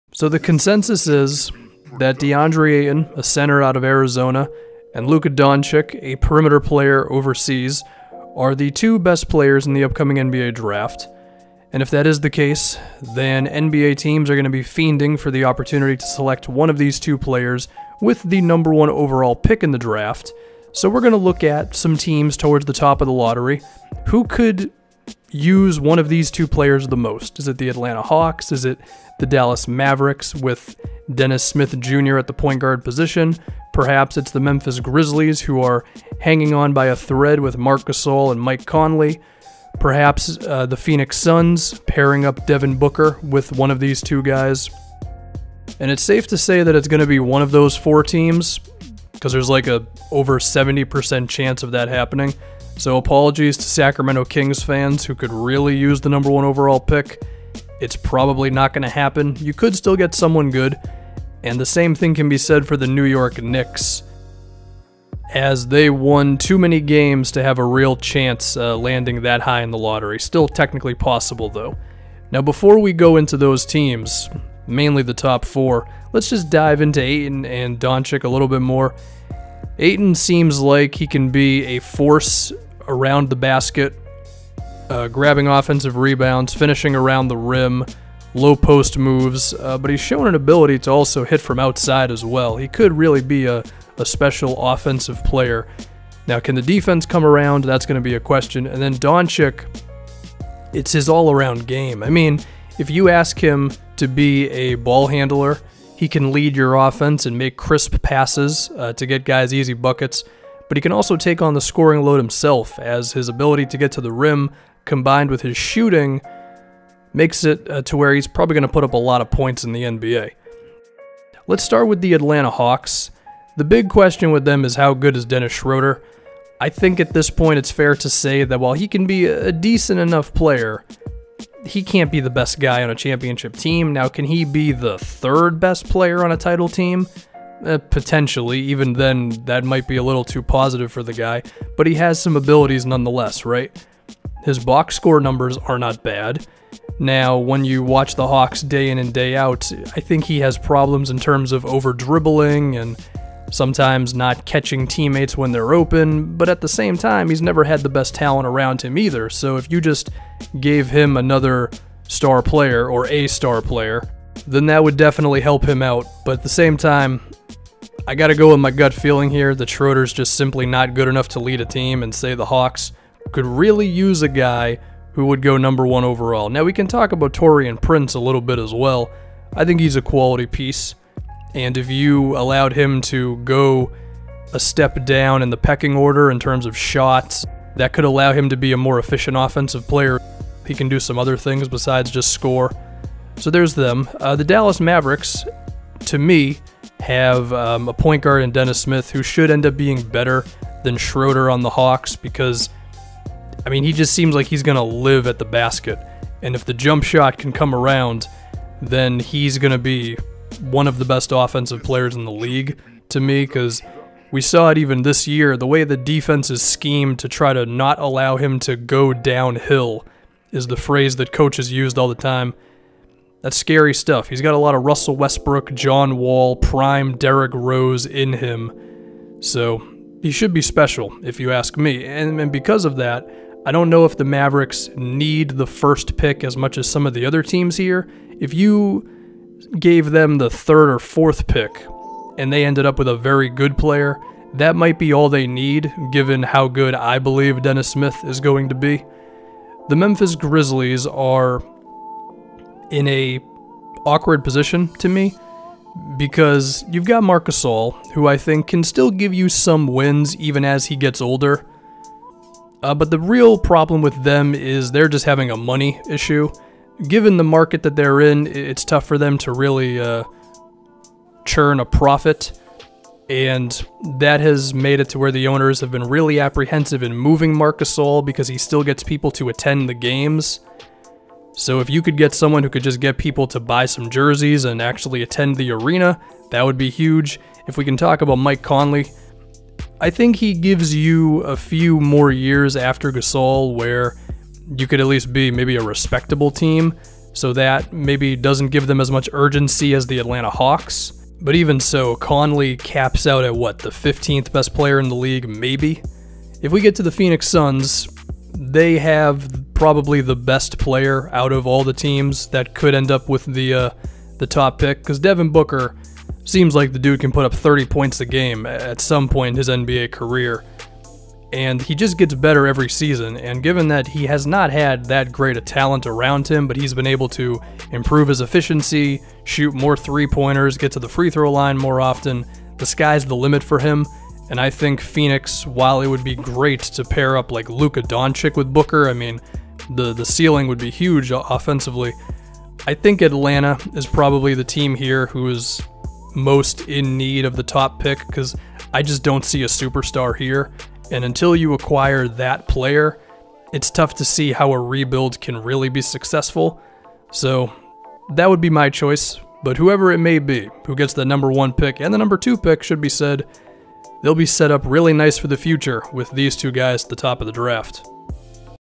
Which NBA Team Needs The Number 1 Pick The Most? | 2018 NBA Draft Lottery
One NBA team really needs Deandre Ayton or Luka Doncic